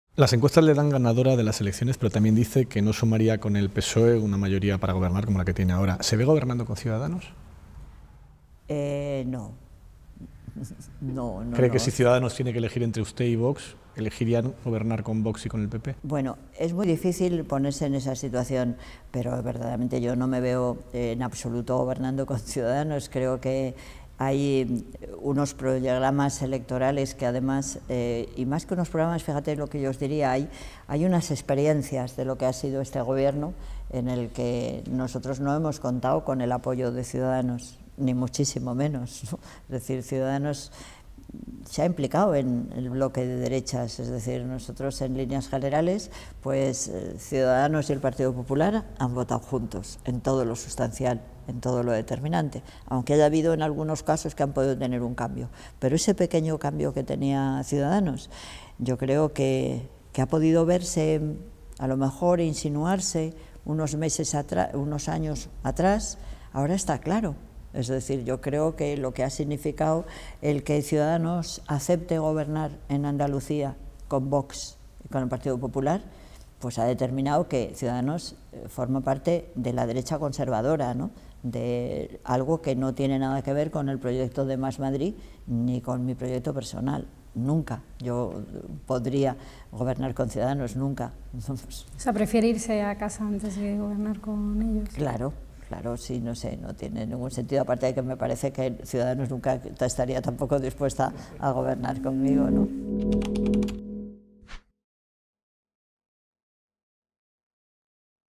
Manuela Carmena: "Yo no me veo gobernando con Ciudadanos"
Manuela Carmena afronta las elecciones como favorita en las encuestas, pero sin que ninguna de ellas le otorgue los apoyos necesarios para poder revalidar la alcaldía. Pese a su experiencia de estos años, reivindica otras formas de afrontar la gestión pública: “La agresividad de la política no responde a una realidad social”.

Apóyanos.